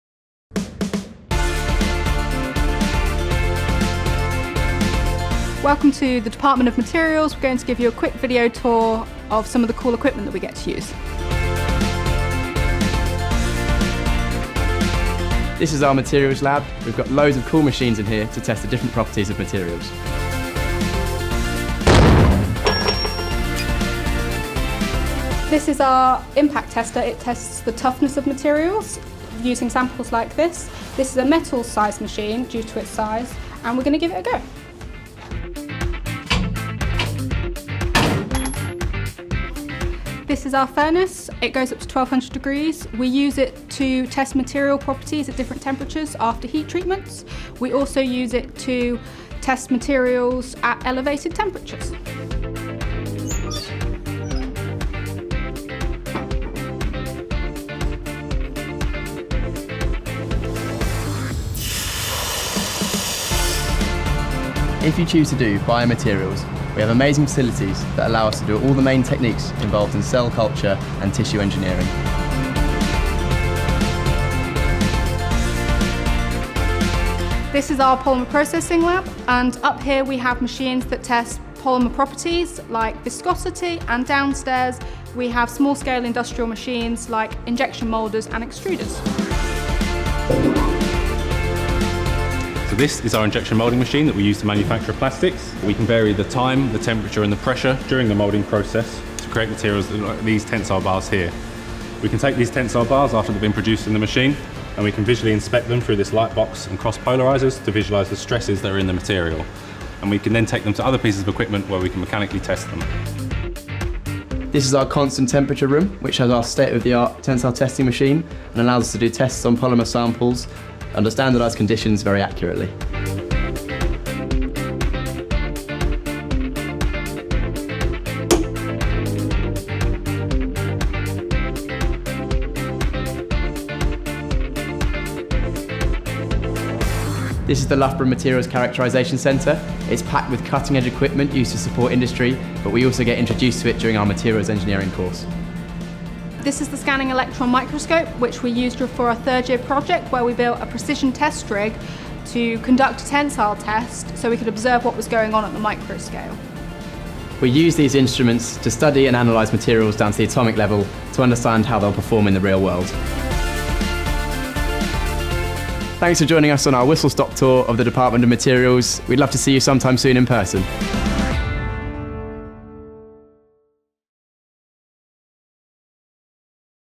Loughborough Materials Engineering Tour
Join some of our Materials Engineering students on a tour of our outstanding facilities, available to those looking to study at Loughborough University.